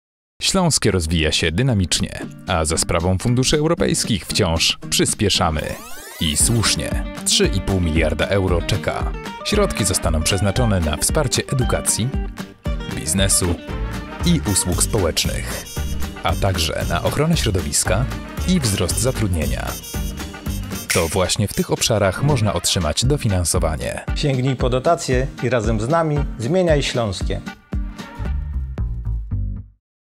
ŚLĄSKIE - PRZYSPIESZAMY! spot 1
Szybko, szybciej, Fundusze! Blisko 3,47 mld euro – taką pulą środków dysponuje Regionalny Program Operacyjny Województwa Śląskiego na lata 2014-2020. Być może część z tej kwoty zostanie przeznaczona na Twój projekt. Zainteresowany? Zainteresowana? Zacznij od strony

Spot z udziałem Marszałka Województwa Ślaskiego Wojciecha Saługi oraz z rysunkami Henryka Sawki, wyprodukowany w ramach kampanii informacyjnej Regionalnego Programu Operacyjnego Województwa Śląskiego na lata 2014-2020 prowadzonej pod hasłem "Śląskie - przyspieszamy!" w 2017 roku na zlecenie Urzędu Marszałkowskiego Województwa Śląskiego.